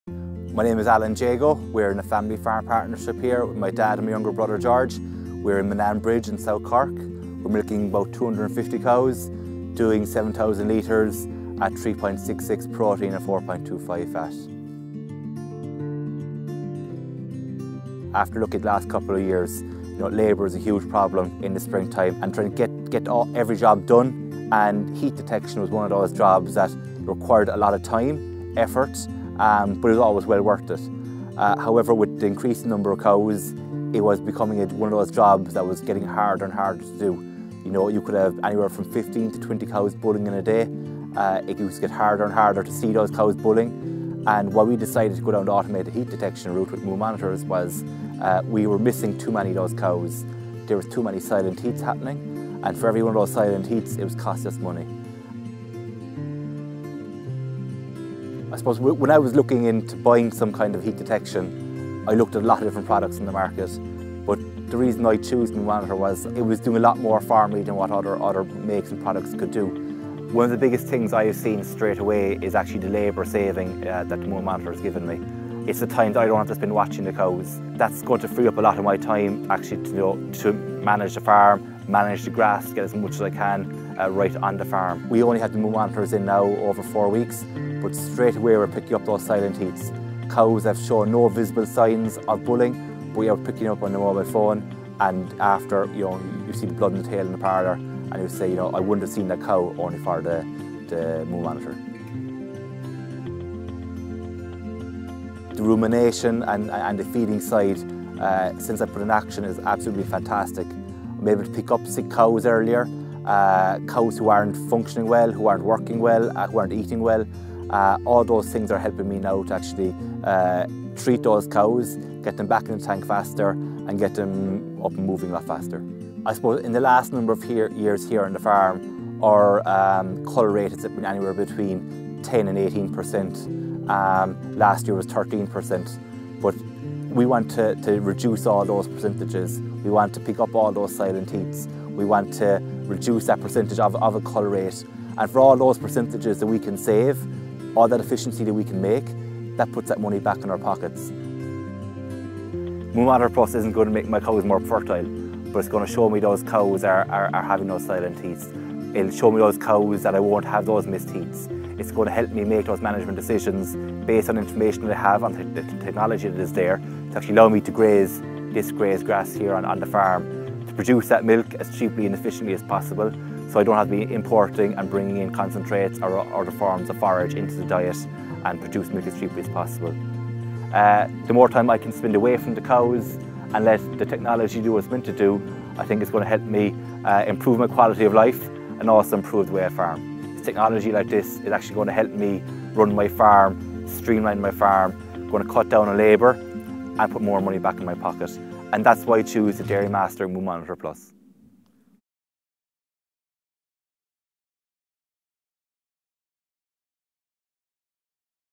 Alan Jagoe discusses farm health and fertility using MooMonitor+
As cow numbers increased dairy farmer Alan Jagoe found it hard to keep up with all the demands of farming. Finding time to carefully monitor cow health and fertility was often difficult so farm production and profit suffered.

Now with Dairymaster's revolutionary MooMonitor+ the heat cycles and health status of his cows are monitored constantly using technology. Alan is informed automatically of important health and fertility events at the right time leaving him to get on with other matters on the farm.